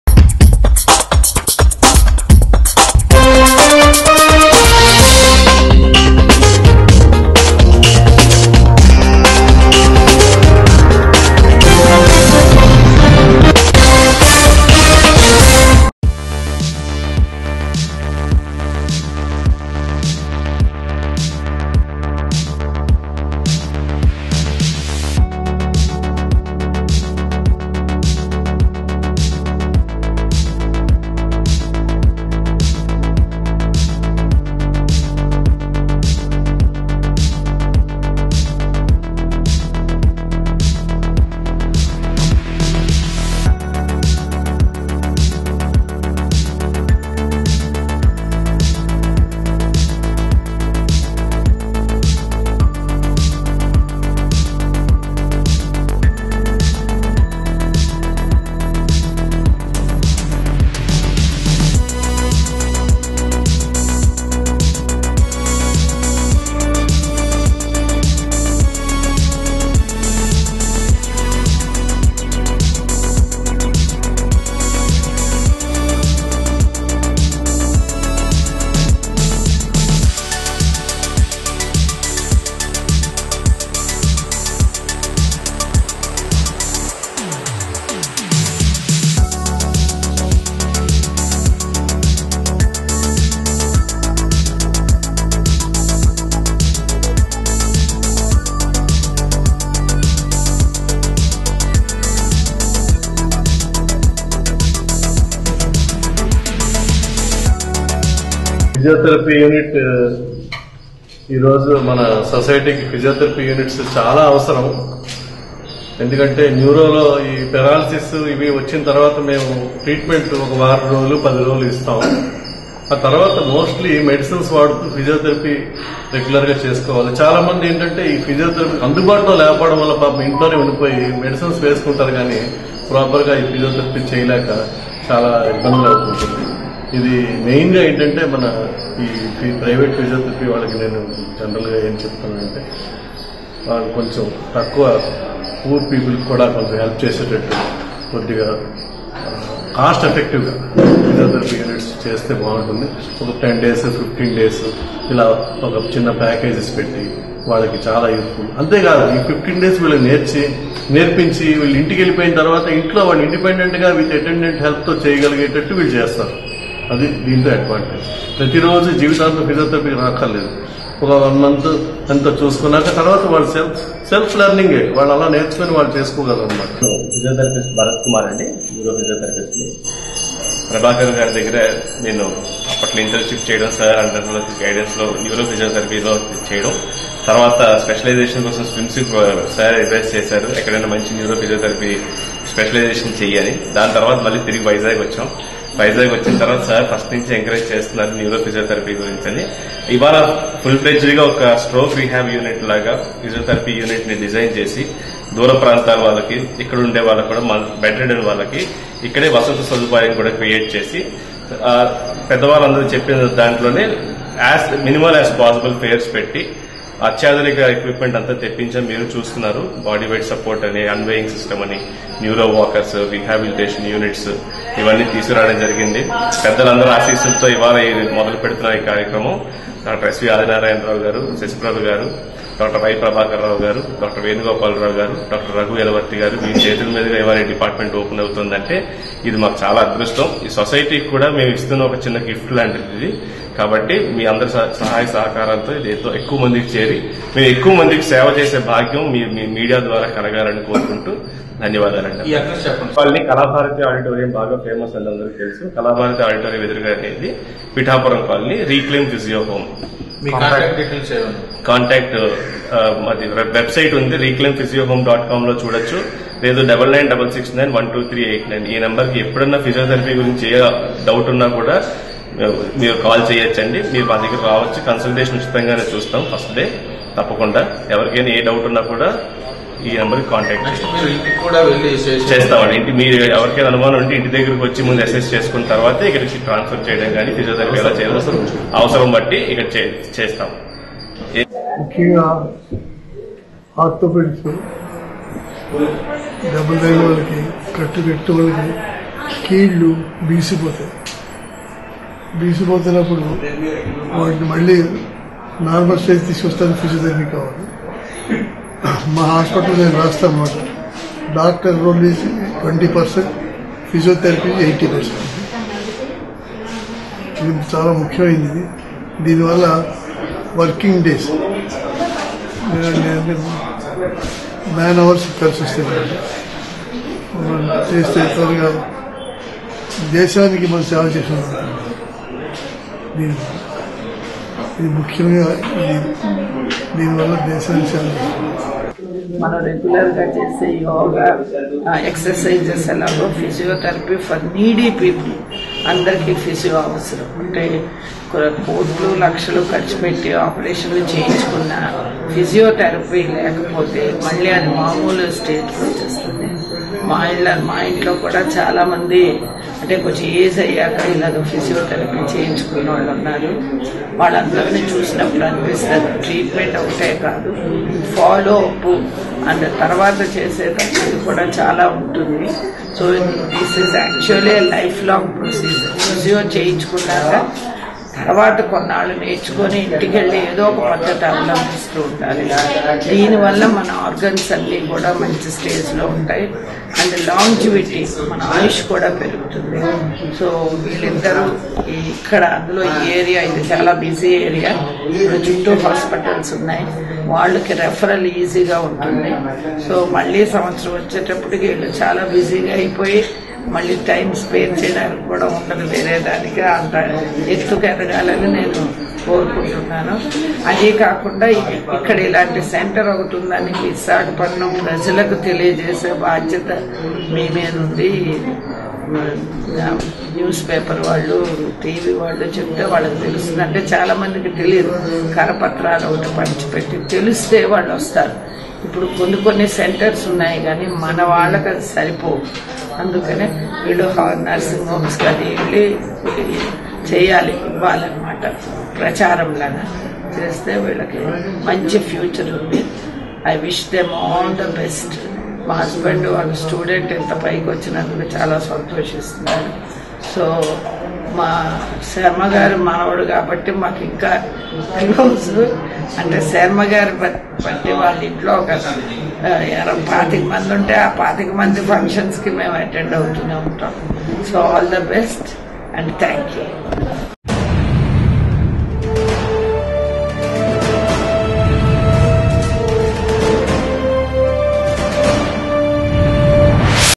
RECLAIM PHYSIOHOME | Inpatient Neuro Physiotherapy & Rehab Center
నేటి ఆధునిక యుగంలో మానవులు ఎన్నో రుగ్మతలతో సతమతమవుతున్నారని  ప్రధానంగా పెరాలసిస్, కాళ్లు నొప్పులు, ఒంటి నొప్పులు, మెడ నొప్పులు తదితర రోగాల బారిన పడి ఇబ్బందులు పడుతున్న వారికి ఉపశమనం కోసం ఫిజియోథెరపీ  ప్రముఖ స్థానం పోషిస్తోందని సీనియర్ వైద్యులు డాక్టర్. ఆదినారాయణ, డాక్టర్. ససి ప్రభ లు పేర్కొన్నారు. మద్దిలపాలెం లోని పిఠాపురం కాలనీ లో రీ క్లయిమ్ ఫిజియో హోమ్ సెంటర్ ను ప్రారంభించిన అనంతరం వారు మాట్లాడుతూ అన్ని వర్గాల వారికి అందుబాటులో ఉండే విధంగా ఫిజియోథెరఫీ చికిత్సను అందివ్వాలని ఫిజియోథెరపీ సెంటర్ మేనేజింగ్ డైరెక్టర్, ప్రముఖ న్యూరో ఫిజియోథెరపిస్ట్ డాక్టర్. భరత్ కుమార్ కు సూచించారు. అనంతరం భరత్ కుమార్ మాట్లాడుతూ తమ ఫిజియోథెరపీ సెంటర్ లో విశేష అనుభవం ఉన్న వైద్యులు ఉన్నారని పేర్కొన్నారు. కొన్ని సందర్భాలకు అనుగుణంగా రోగులకు ఇంటి వద్దనే  తమ ఫీజియోథెరపీ సేవలు అందించేందుకు తాము సిద్ధంగా ఉన్నామని తెలిపారు. ఈ కార్యక్రమంలో  ఫిజియోథెరపిస్ట్ డాక్టర్. శేష శైలజ నగరానికి చెందిన ప్రముఖ వైద్యులు తదితరులు  పాల్గొన్నారు.